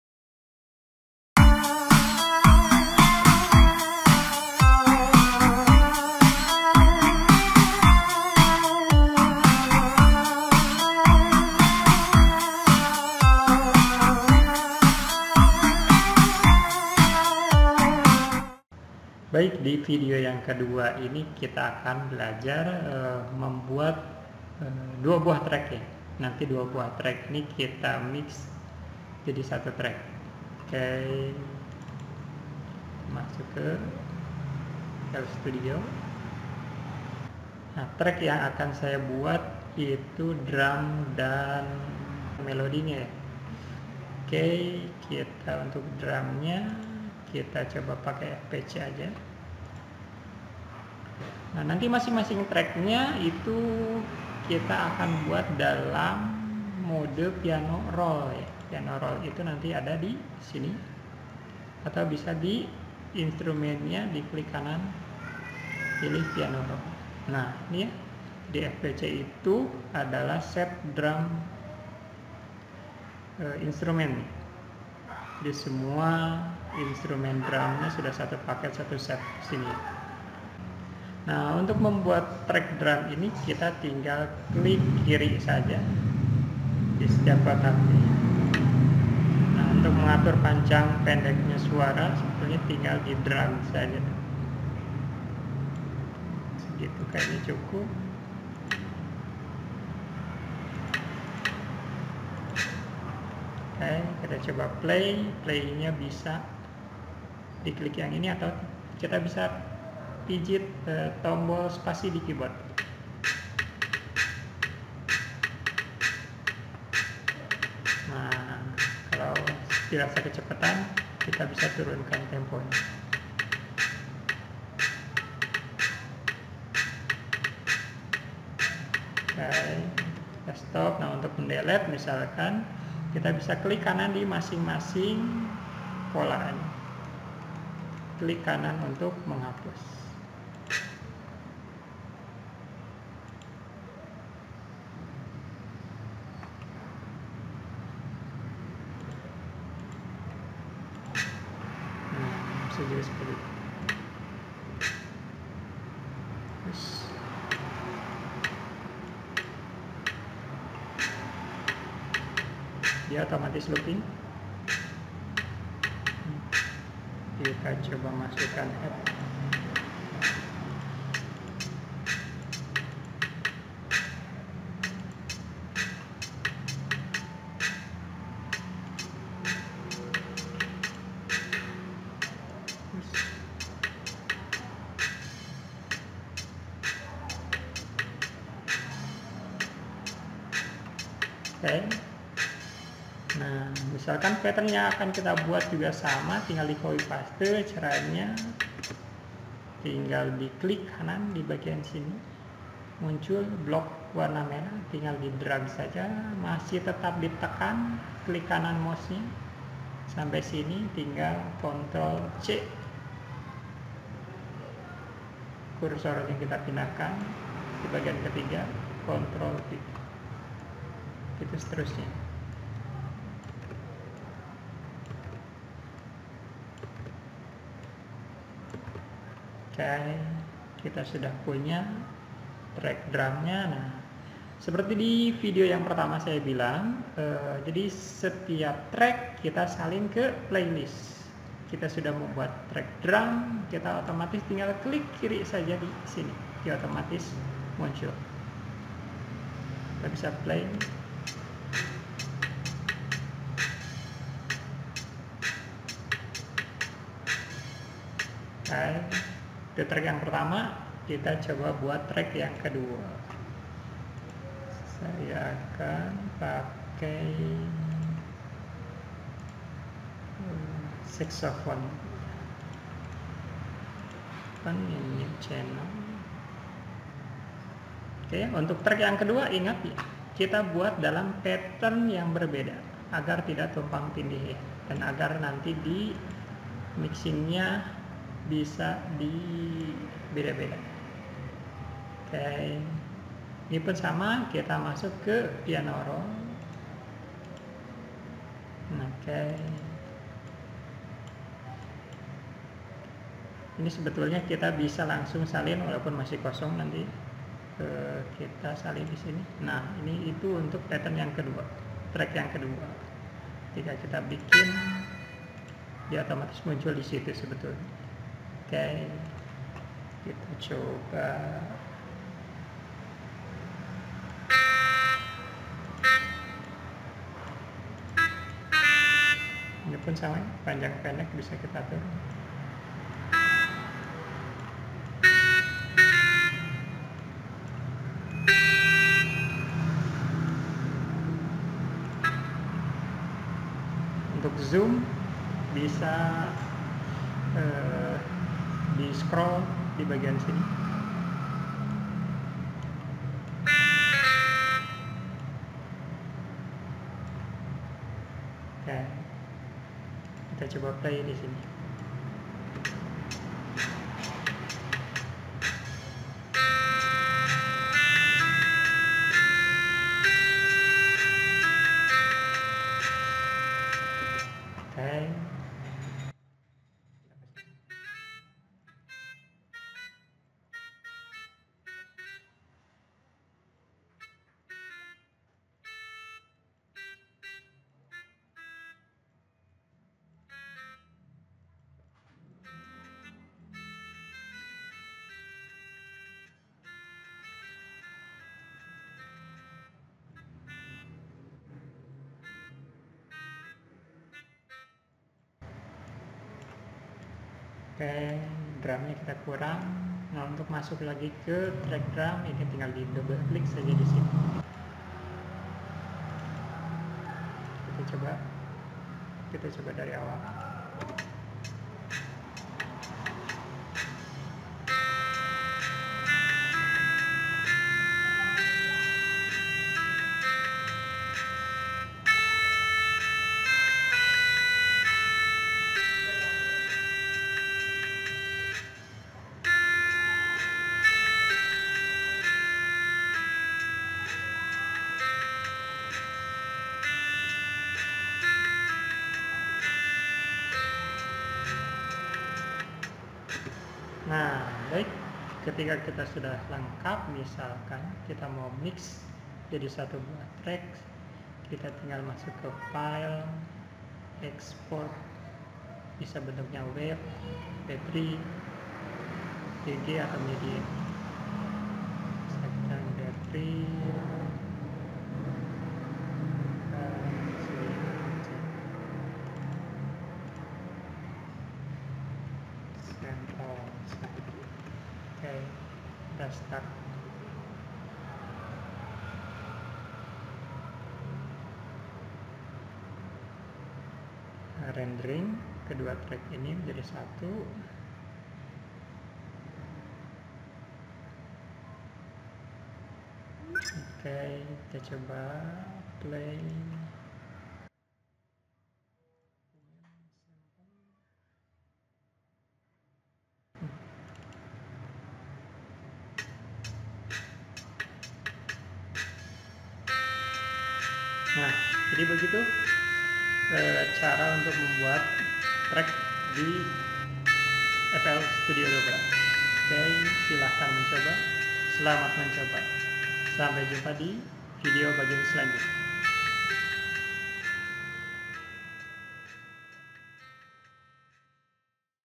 Tutorial FL Studio part 2
Membuat track drum (dengan FPC) dan track melody, kemudian di mix